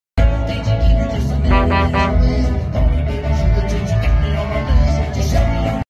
Georgina Leahy Honking Breast at Concert
Actres & model Georgina Leahy honks her right breast at a concert.  Sound effect added for convenience.